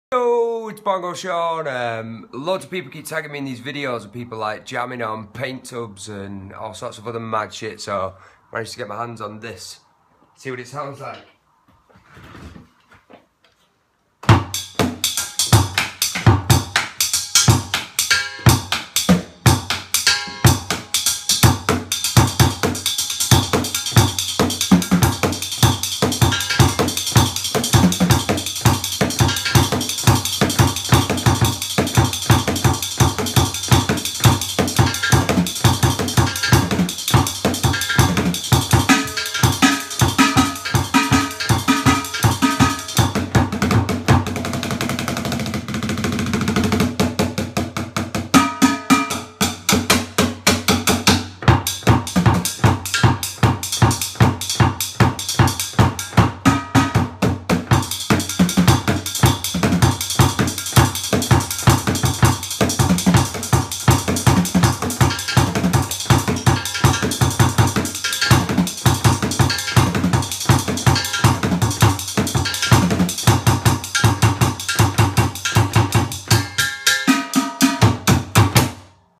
Drumkit made out of paint tubs and garbage cans
Rockin' out on a makeshift drumkit.